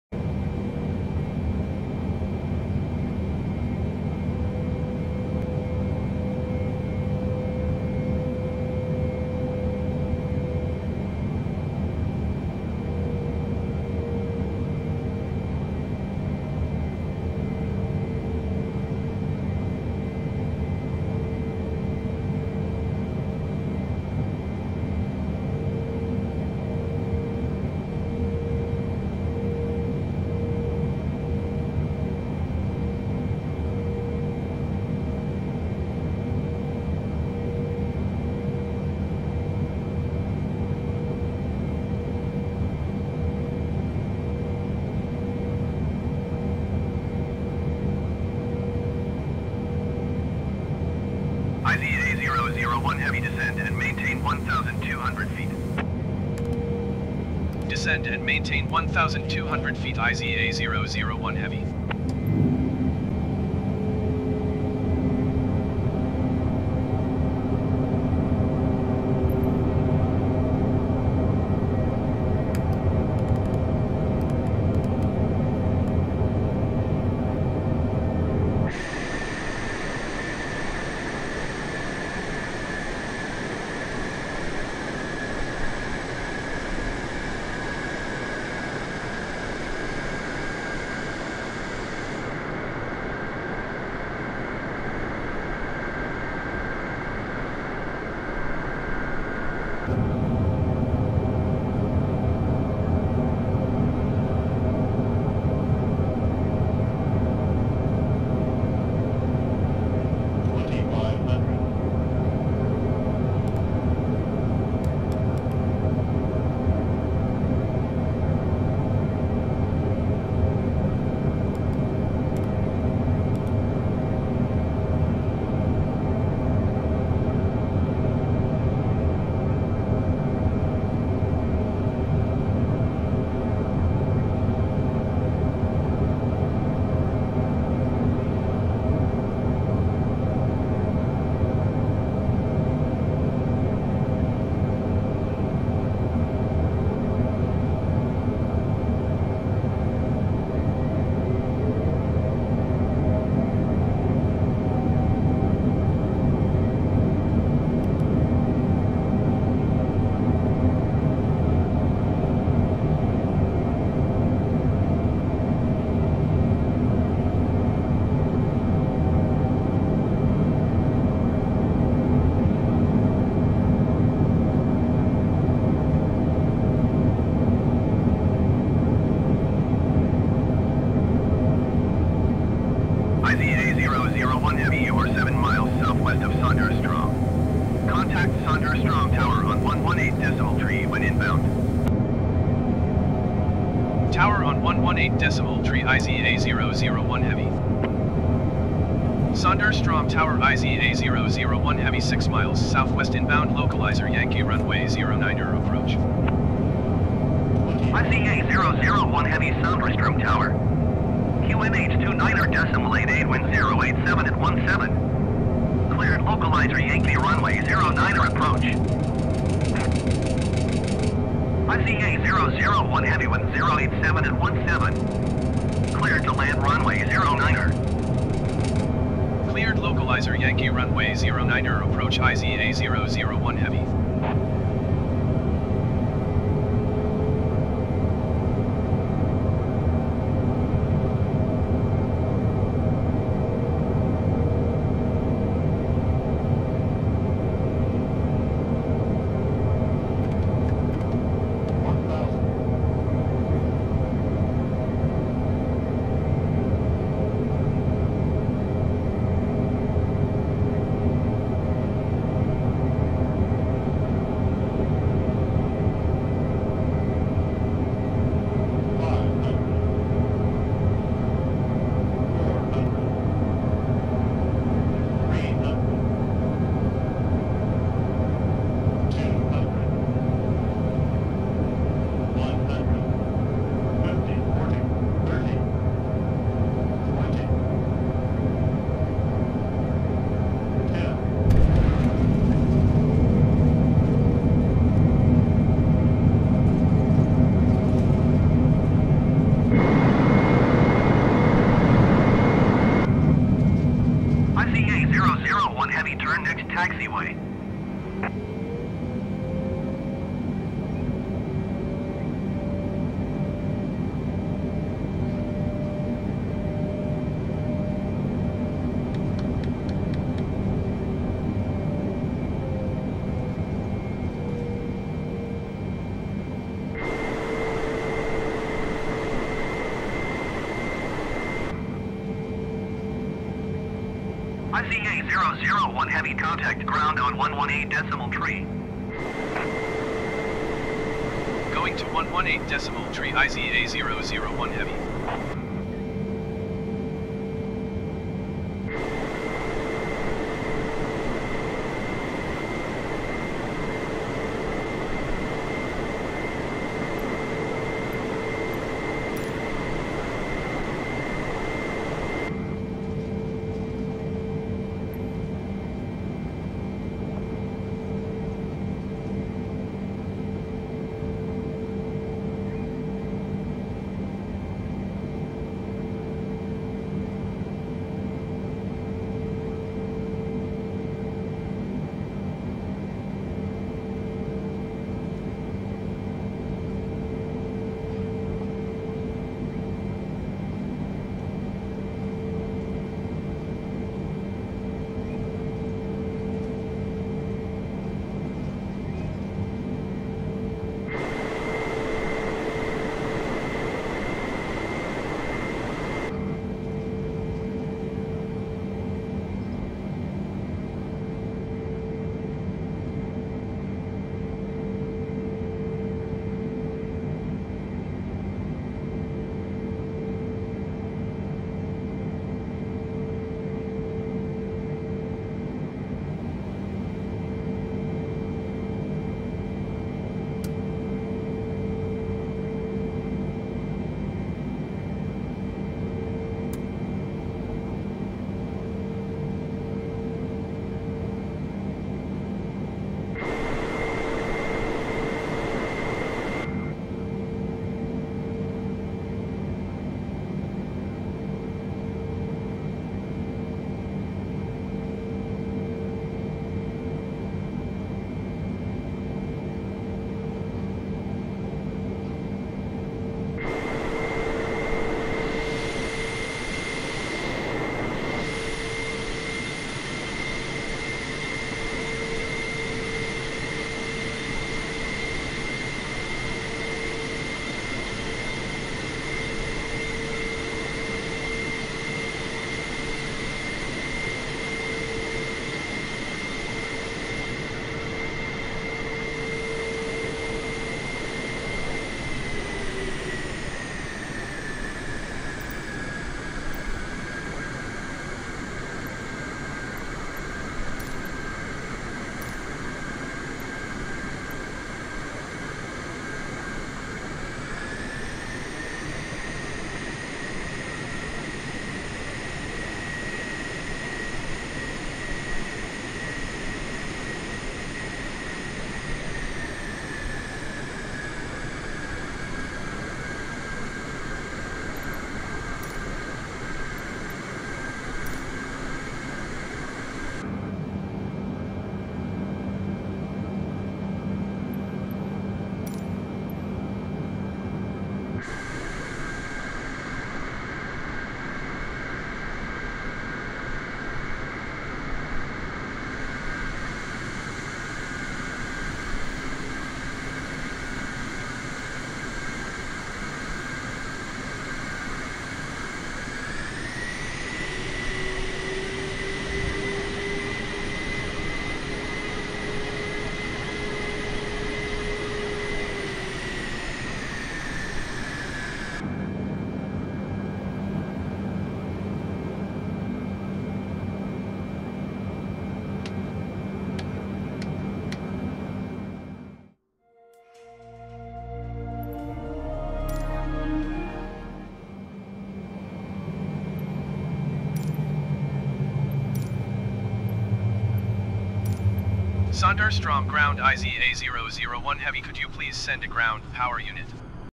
747-8 landing at BGSF
Came in a bit high and fast, but made it in one piece.